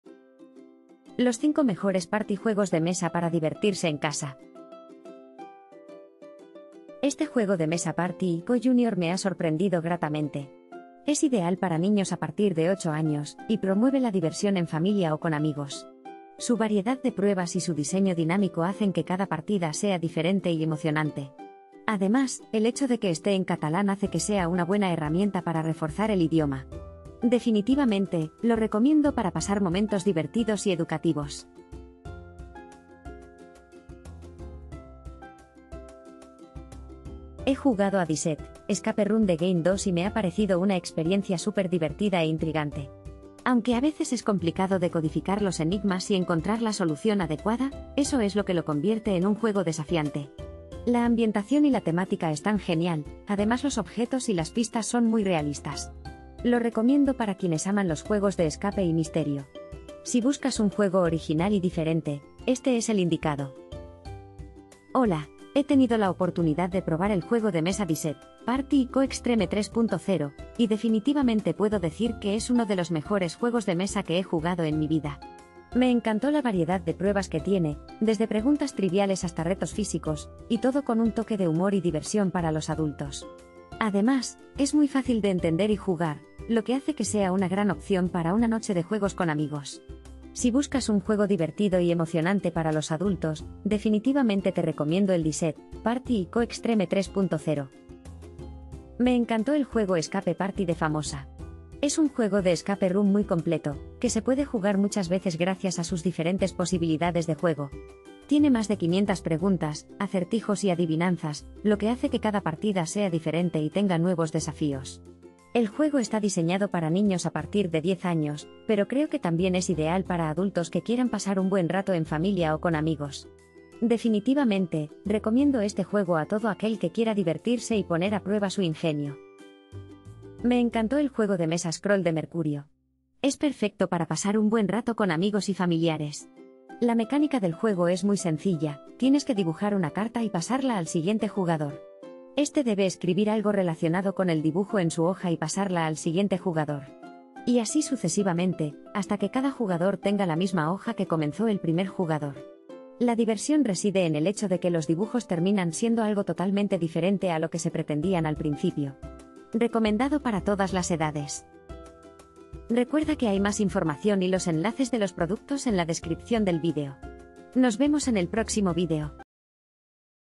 Party juego de mesa: Los 5 mejores party juegos de mesa para divertirse en casa
✋  Aquí están los 5 mejores party juegos de mesa para divertirse en casa disponibles en el mercado.

🔽🔽🔽 ENLACES DE COMPRA PARTY JUEGO DE MESA 🔽🔽🔽

1) Diset - Juego Party & co Junior, Juego de mesa infantil multiprueba a partir de 8 años en catalán

2) Diset - Escape room the game 2, Juego de mesa adulto a partir de 16 años

3) Diset - Party & Co Extreme 3.0, Juego de mesa multiprueba adulto a partir de 16 años

4) Famosa – Escape Party, juego de Escape Room muy completo, con posibilidades de juego diferentes para jugar muchas veces, hasta 500 preguntas, acertijos y adivinanzas, a partir de 10 años, (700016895)

5) Mercurio Juego de Mesa Scrawl (BP0003), 8 years and above

00:00 - Intro
00:05 - 1) Diset - Juego Party & co Junior, Juego...
00:36 - 2) Diset - Escape room the game 2, Juego...
01:03 - 3) Diset - Party & Co Extreme 3.0, Juego...
01:37 - 4) Famosa – Escape Party, juego de Escape Room...
02:09 - 5) Mercurio Juego de Mesa Scrawl (BP0003), 8 years...
02:41 - Outro